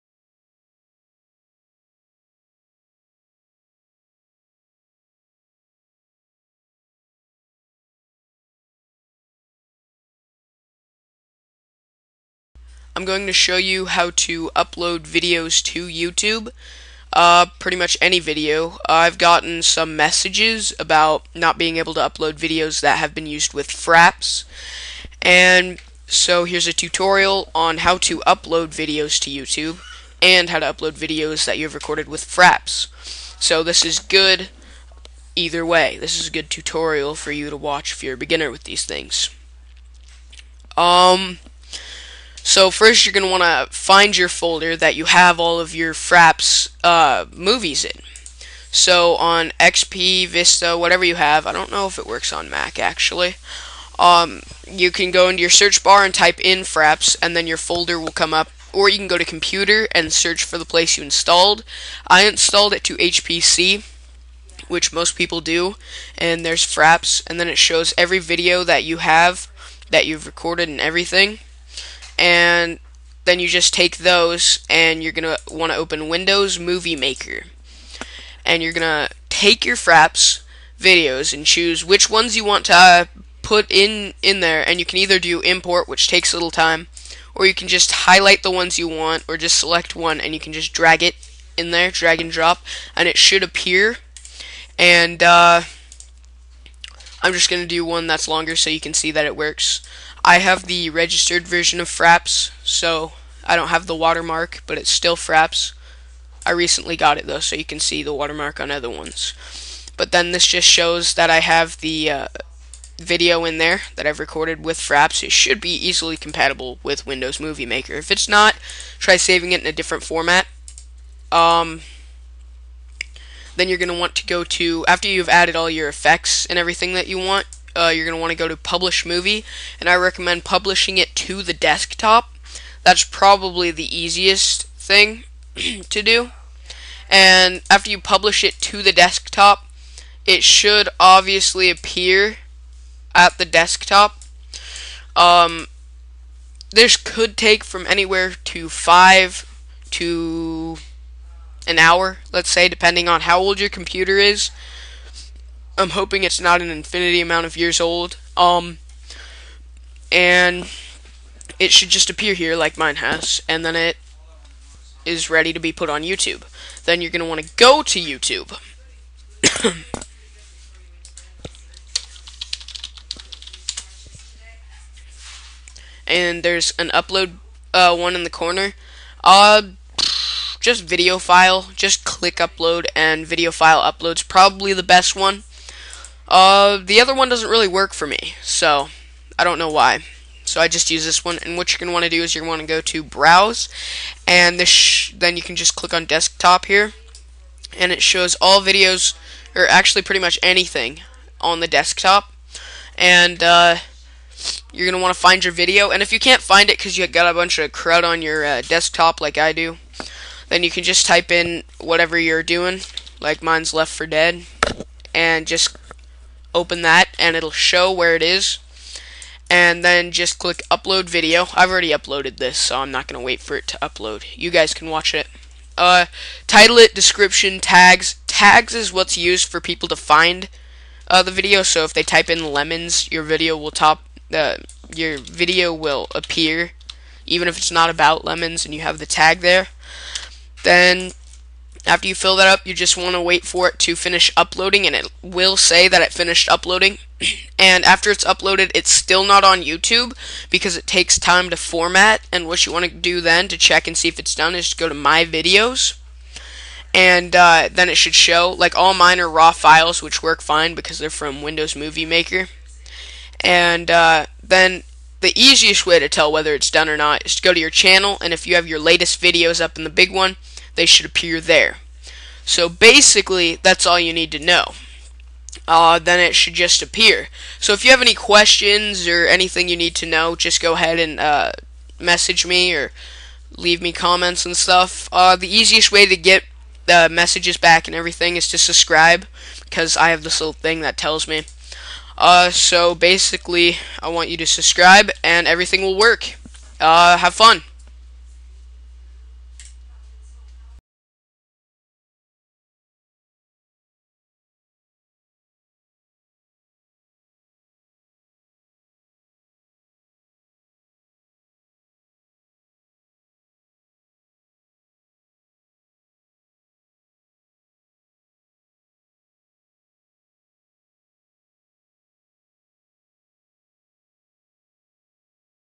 How to upload videos to youtube (fraps included)
Helping with uploading if you don't know how watch this if you have questions please ask.